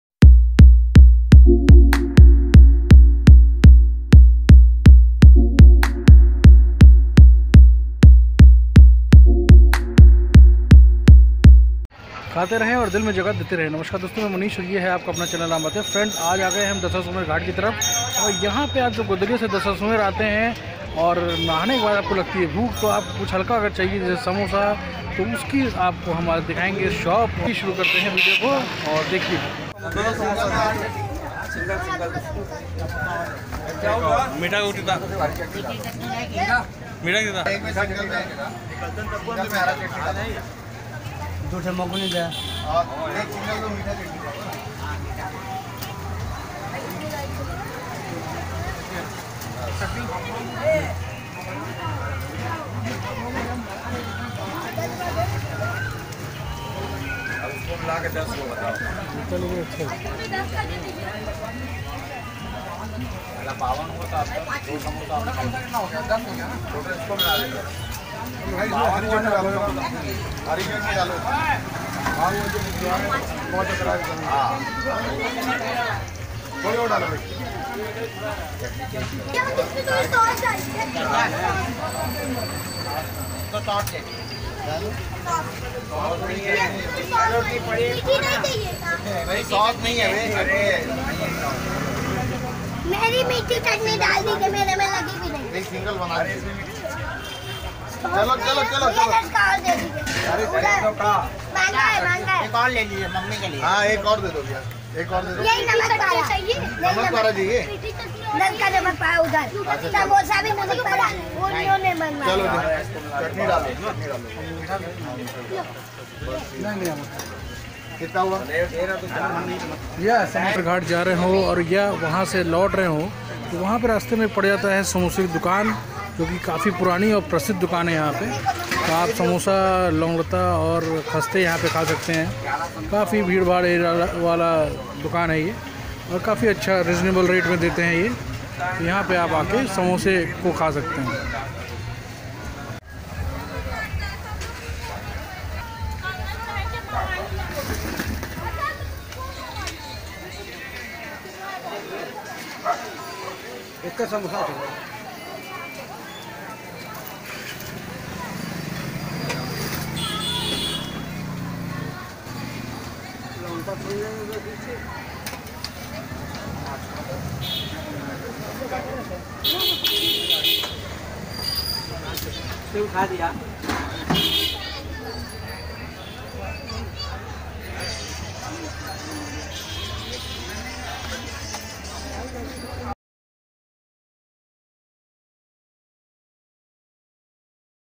दशाश्वमेध का फेमस समोसा / Varanasi
Is video mein aap dekenge famous Shop jaha nasata milta hain.